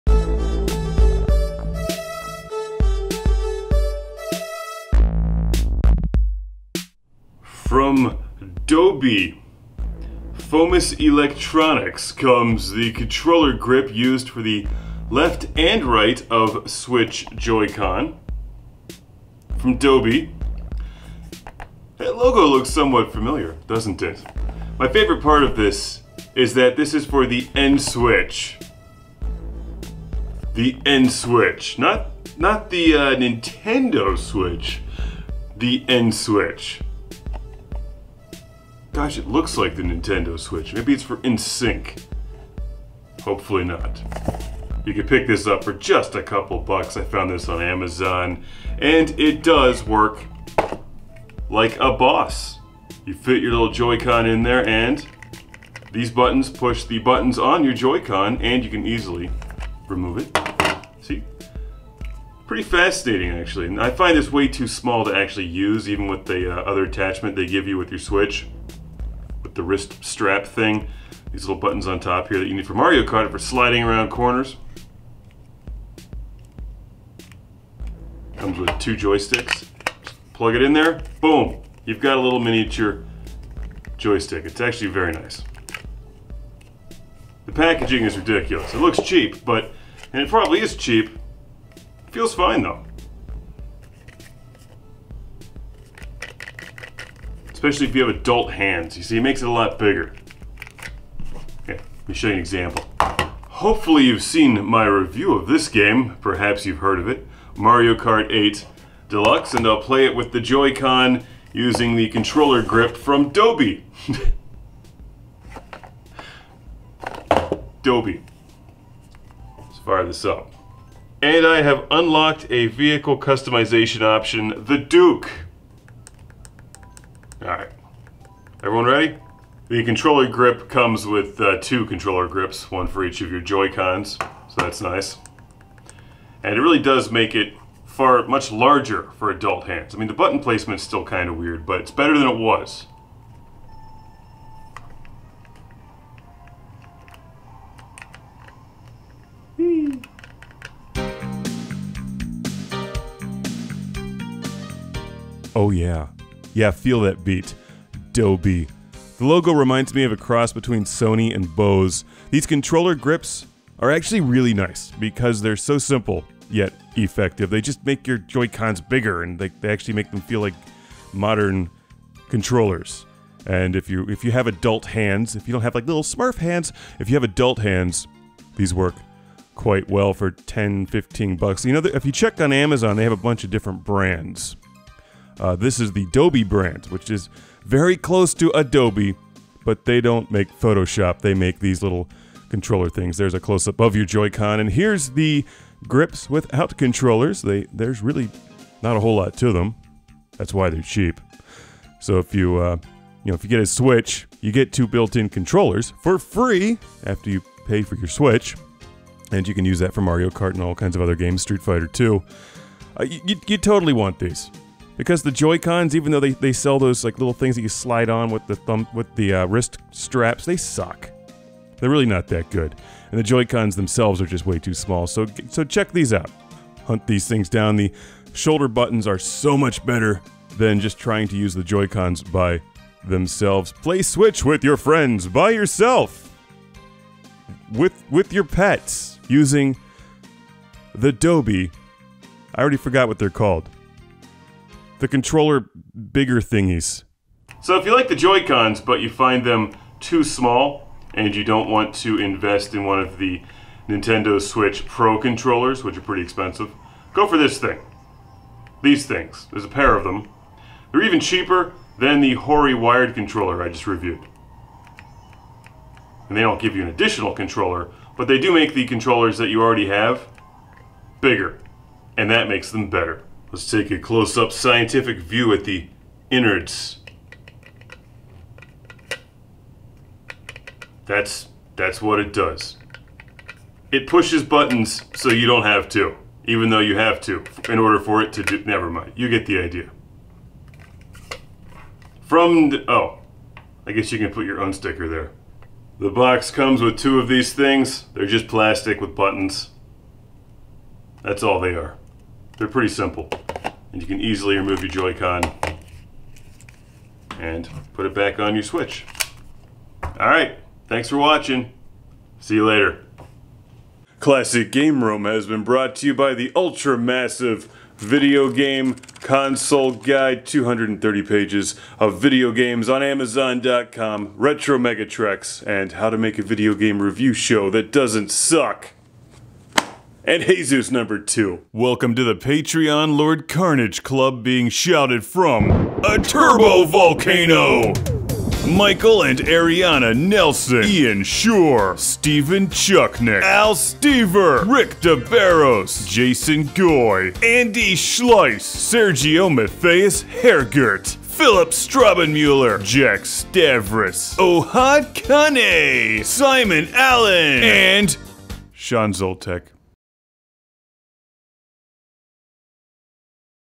Classic Game Room - NINTENDO SWITCH CONTROLLER GRIP review (DOBE)
ULTRA MASSIVE VIDEO GAME CONSOLE GUIDE VOLUME 1

Classic Game Room reviews the NINTENDO SWITCH CONTROLLER GRIP from Dobe, a controller grip that you add to the Nintendo Switch Joy-Con to make it bigger and better. CGR Nintendo Switch controller grips review has Dobe NIntendo Switch controller grips that you plug your Nintendo Switch Joycon into.